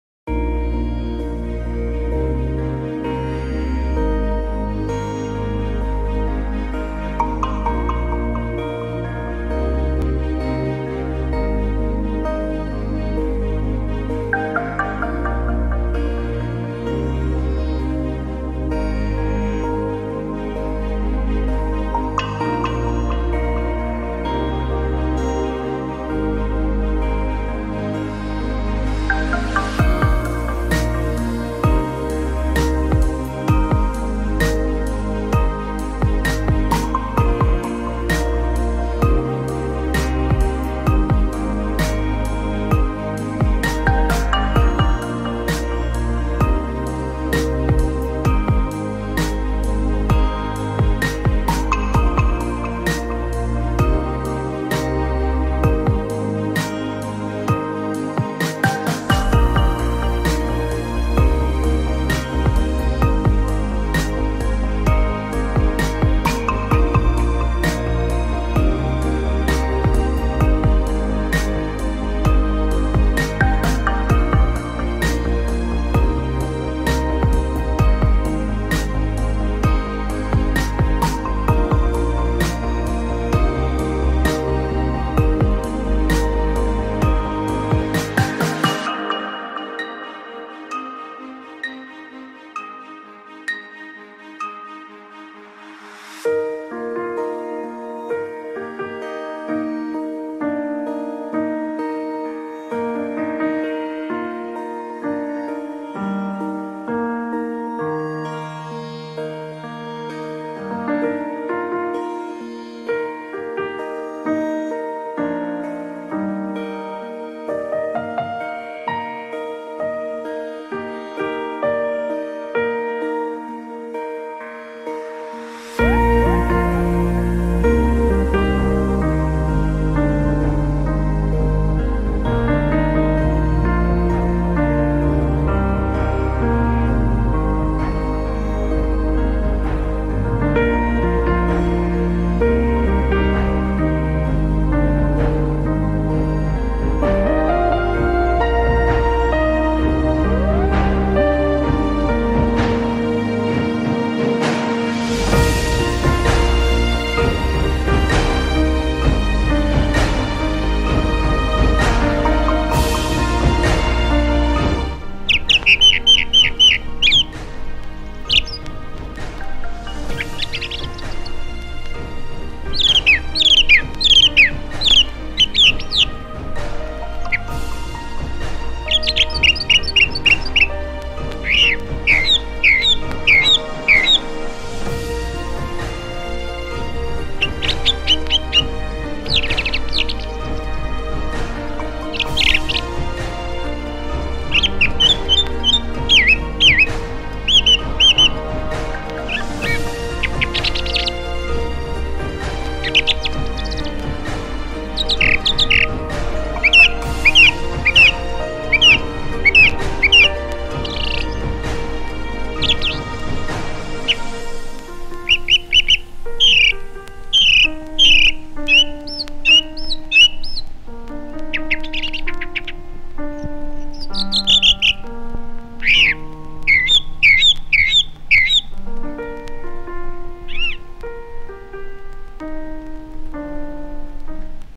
The Worm Murderer in our garden
We are lucky to have an orange and a grapefruit tree in our garden which a New Zealand Song Thrush has made its nest and now has a few chicks installed (rent free of course). This movie was taken while having afternoon tea outside and at a distance of approx 25 feet.
This has been my first attempt at bird life.

Birdsong was recorded for the NZBC some many years ago ( I have lots of others if anyone would like them

Movie taken with the sony HDR CX 625  and rendered with Power director 16